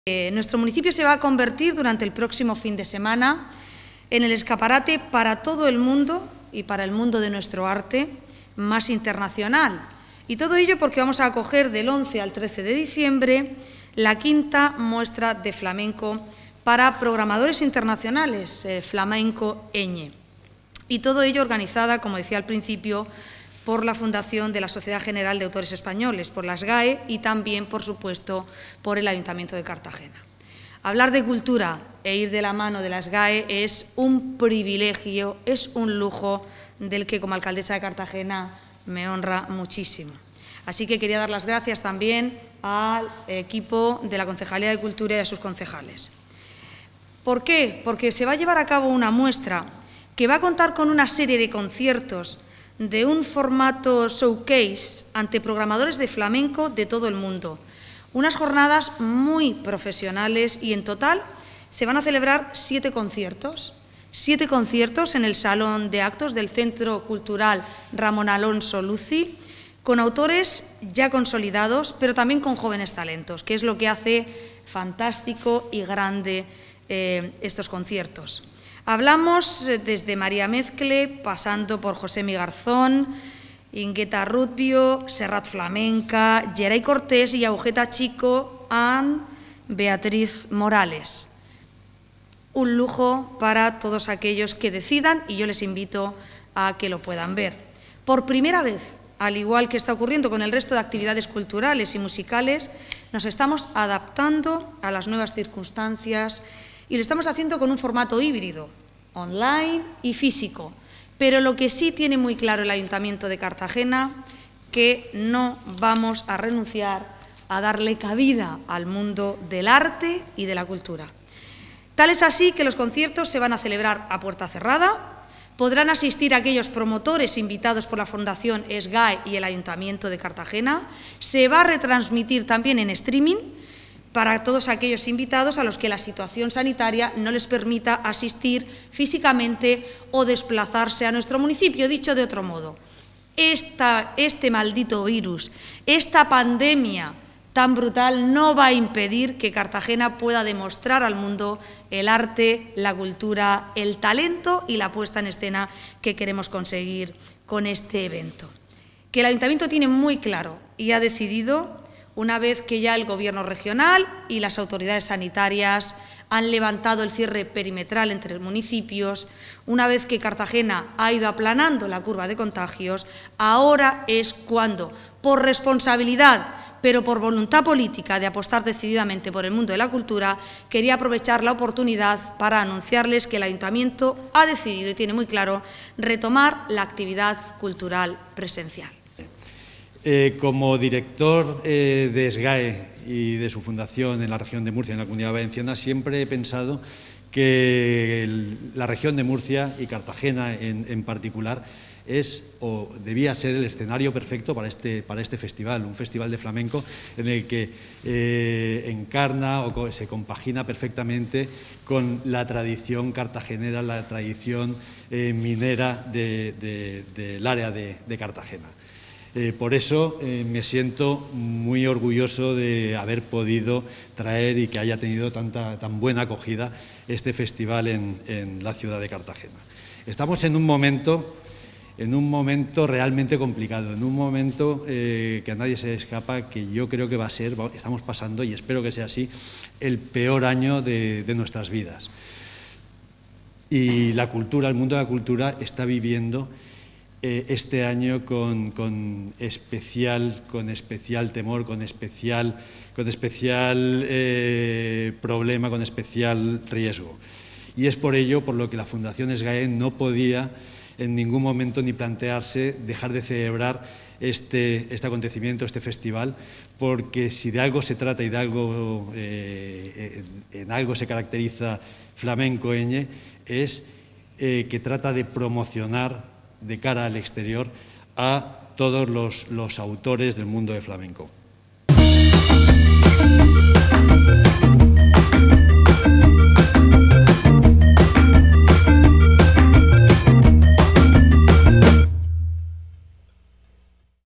El Ayuntamiento de Cartagena retoma la actividad cultural presencial
La programación cultural municipal volverá a contar con público. Así lo ha anunciado la alcaldesa de Cartagena, Ana Belén Castejón, una vez que los datos epidemiológicos continúan mejorando.

La primera edil municipal, acompañada del concejal del área y el concejal delegado de Cultura, David Martínez Noguera y Carlos Piñana, respectivamente, ha trasladado esta decisión durante la presentación de la 5ª Muestra de Flamenco para Programadores Internacionales, Flamenco Eñe.

“Una vez que hemos logrado frenar la curva de contagios e incluso hemos conseguido reducirla, hemos decidido que la cultura vuelva a contar con público. Eso sí, garantizando siempre todas las medidas sanitarias recomendadas”, ha señalado la alcaldesa.

“En breve, daremos a conocer la programación que hemos preparado de cara a Navidad, que incluirá representaciones teatrales y musicales, entre otras”, ha indicado la primera edil municipal.

Castejón ha subrayado que “Cartagena es un municipio vivo, que apuesta por la Cultura y que va a seguir haciéndolo”.

5ª Muestra de Flamenco Eñe

El flamenco será el gran protagonista durante el próximo fin de semana. Desde el 11 hasta el 13 de diciembre, Cartagena acogerá la 5ª Muestra de Flamenco para Programadores Internacionales, Flamenco Eñe, organizada por la Fundación de la Sociedad General de Autores Españoles (SGAE) y el Ayuntamiento de Cartagena.

El festival estará formado por siete conciertos en formato ‘showcase’ ante programadores de flamenco de todo el mundo y unas jornadas profesionales. Se realizarán tanto de forma online como presencial pero serán a puerta cerrada, contando con la presencia de los promotores invitados por la fundación SGAE y el Ayuntamiento de Cartagena.

“También se retransmitirá vía streaming para aquellos invitados a los que la situación sanitaria no les permita desplazarse. El público podrá disfrutar de estos conciertos gracias a la colaboración de TeleCartagena que lo retransmitirá la próxima semana”, ha señalado la alcaldesa, Ana Belén Castejón.

“Este festival es un lujo para aquel que pueda verlo”, ha manifestado Castejón.

Por su parte, el director de la fundación SGAE en la zona de Valencia y Murcia, Álvaro Oltra, ha explicado que “desde la SGAE estamos muy agradecidos por la acogida que ha tenido en Cartagena este festival del que nos sentimos tan orgullosos. Desde su creación, sabíamos que Cartagena debía ser su escenario porque se corresponde perfectamente con la tradición minera de esta zona”.

“Estamos pasando por el peor año de nuestras vidas y el mundo de la cultura lo está viviendo con especial riesgo. Por eso, no podíamos ni plantearnos no hacer este festival, que busca promocionar a los autores del mundo del flamenco y fomentar su participación en giras internacionales. Las cuatro ediciones anteriores han sido un tremendo éxito y estoy segura de que ésta también lo va a ser”, ha trasladado Álvaro Oltra.

El festival Flamenco Eñe estará formado por siete conciertos que se celebrarán en el Salón de Actos del Centro Cultural Ramón Alonso Luzzy y contará con autores como María Mezcle, Josemi Garzón Quartet, Ingueta Rutbio, Serrat Flamenca, Yerai Cortés y Agujeta Chico and Beatriz Morales.